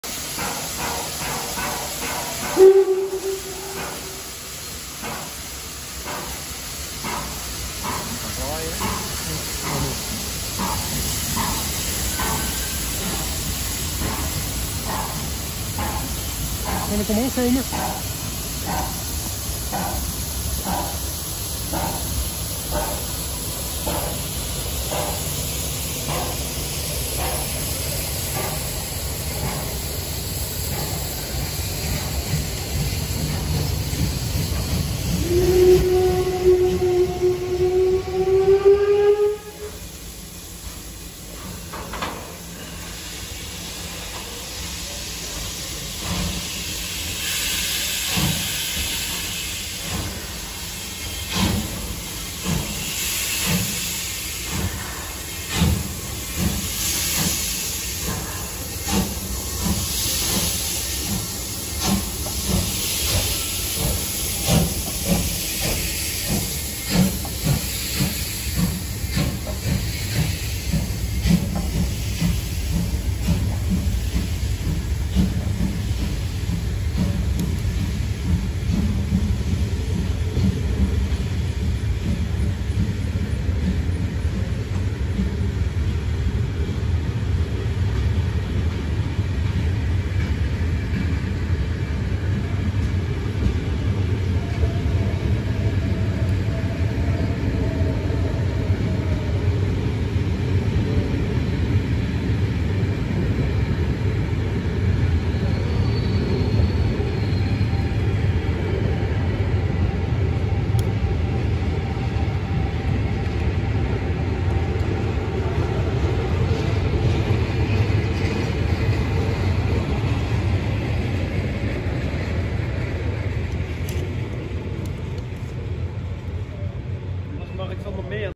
Stoomtrein Stichting Nederland (SSN) Reünie Express vertrekt S’avonds uit Amersfoort Centraal!
Foto’s en Video’s zijn door MIJ gemaakt. Als u een van deze video’s of foto’s gebruikt zonder mijn toestemming dan rapporteer ik het aan de juiste instanties. Na meerdere kopierecht-schendingen doe ik mogelijk aangifte